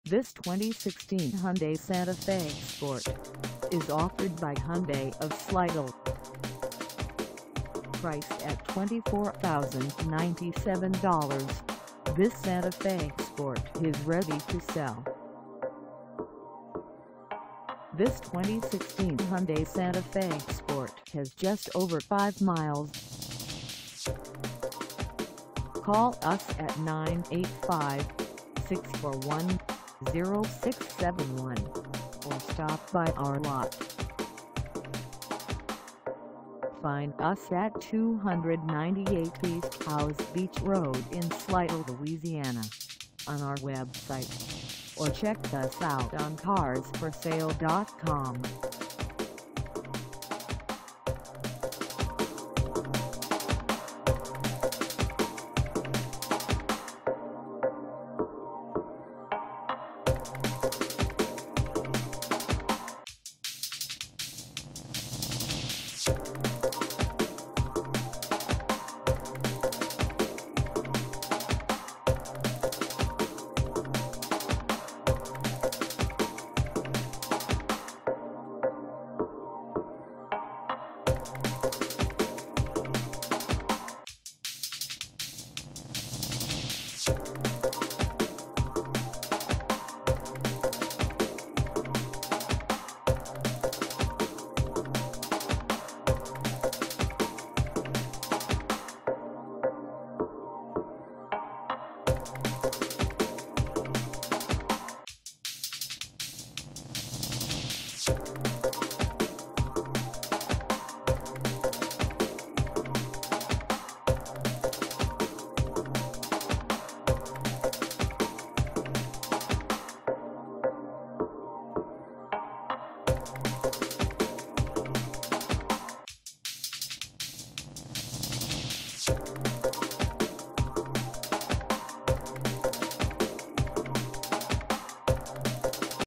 2016 Hyundai Santa Fe Sport for sale in Slidell, LA 70461 at
This 2016 Hyundai Santa Fe Sport is for sale in Slidell, LA 70461 at Hyundai Of Slidell.
Engine: I4 2.4L I4
Transmission: Automatic 6-Speed
Color: Sparkling Silver
Mileage: 5
Contact Hyundai Of Slidell 298 East Howze Beach Road Slidell, LA 70461 to test drive this 2016 Hyundai Santa Fe Sport  today.